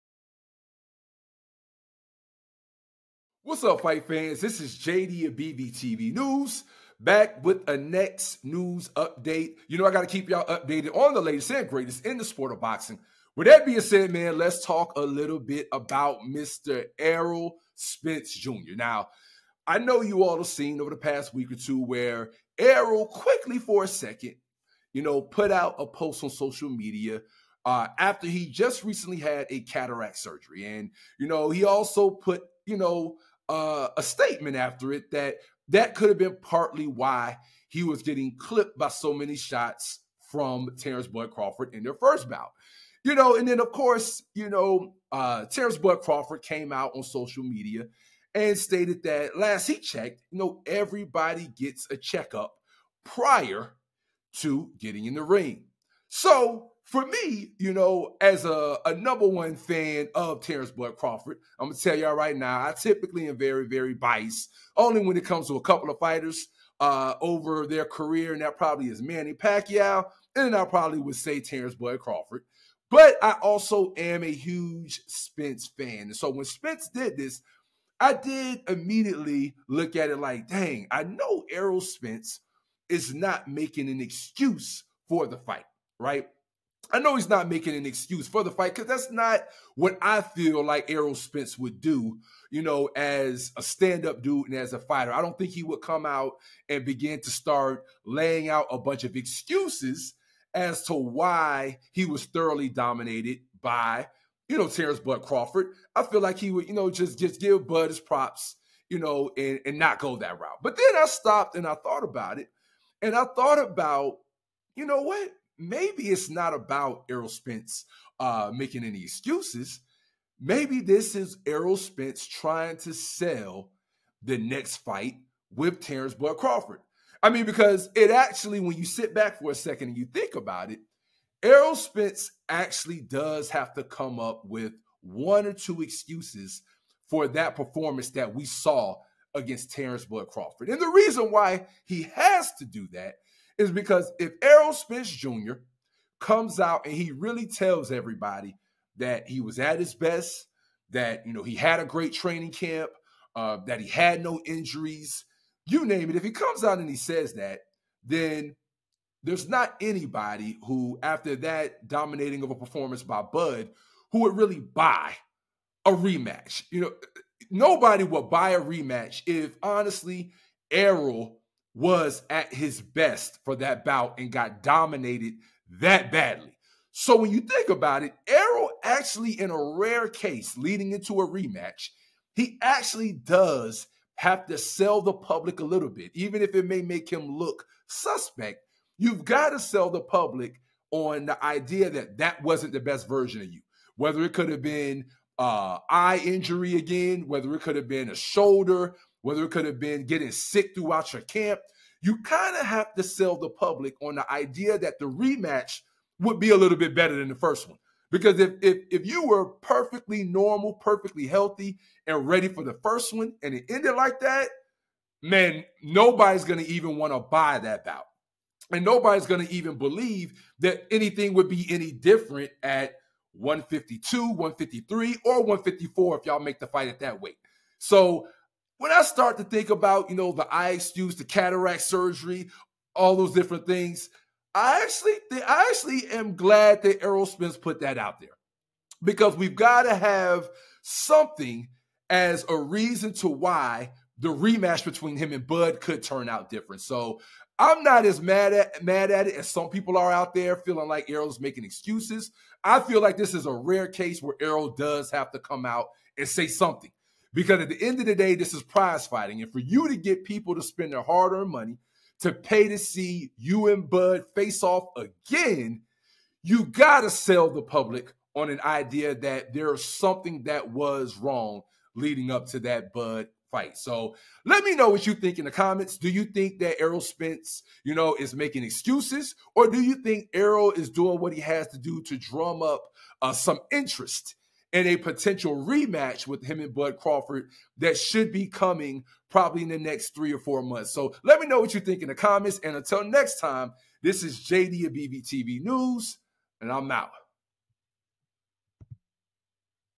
BVTVNEWS: ERROL SPENCE AND TERENCE CRAWFORD DEBATE ERROLS EYE EXCUSE
BVTVNEWS: Errol Spence and Terence Crawford debate the validity of his eye cataracts and what impact it had on their bout.